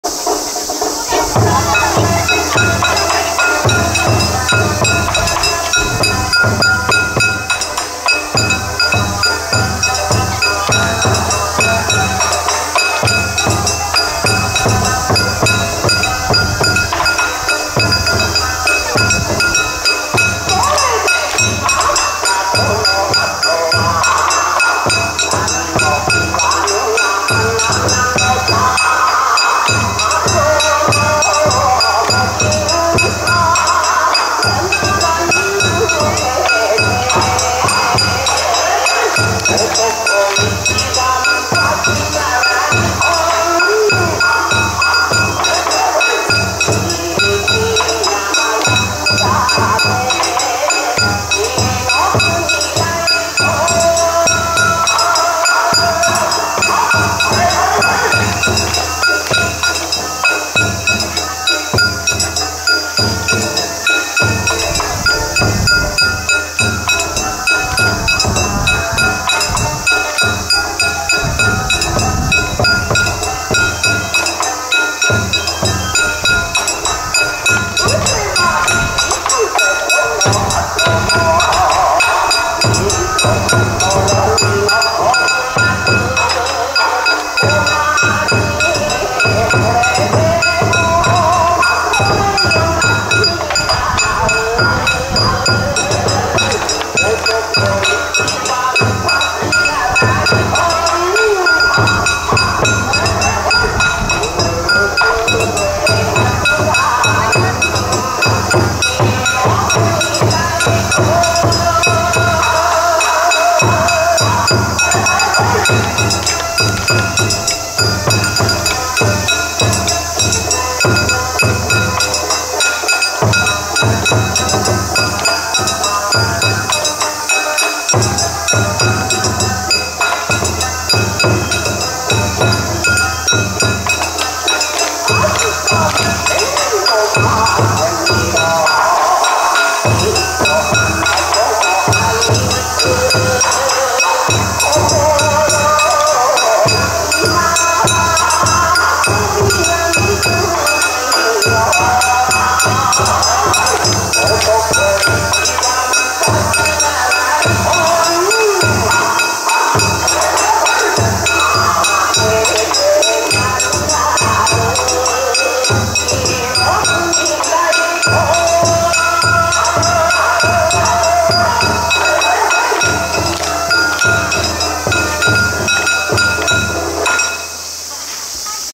４K🏮火の国太鼓🏮西久保八幡神社盆踊り大会2023
地下鉄・神谷町駅近くの西久保八幡神社境内で盆踊り大会2023が華やかに盛大に開催されました。